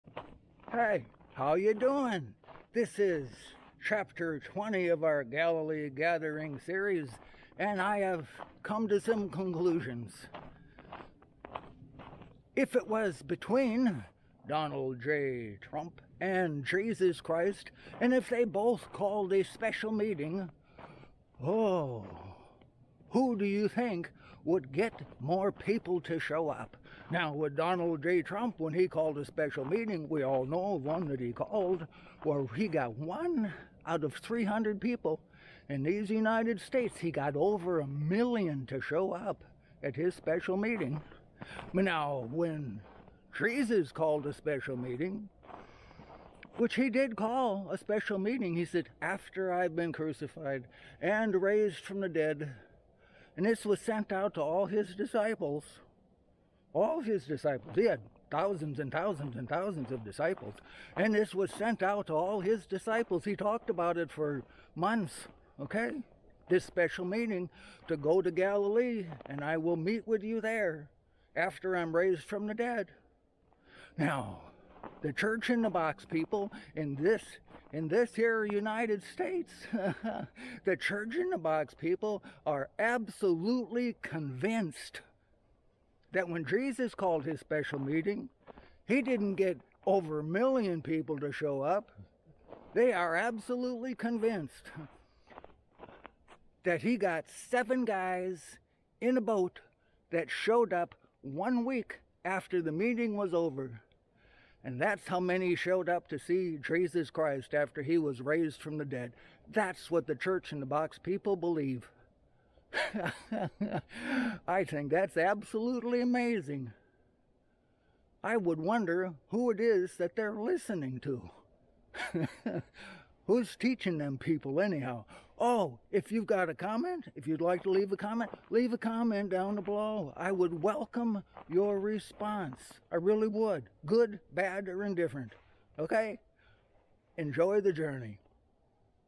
We asked Church going people, who would draw a bigger crowd, Donald J Trump, or Jesus?
Part 20, of "The Galilee Gathering" & The results are in from our Bible understanding 90-day test, observations from around the world. At a specially called meeting, who do they think would proportionally draw a bigger crowd to that meeting, Donald J. Trump, or Jesus Christ? (Considering the US population is way over 300 million and two thousand years ago Israel's population was about 6 million).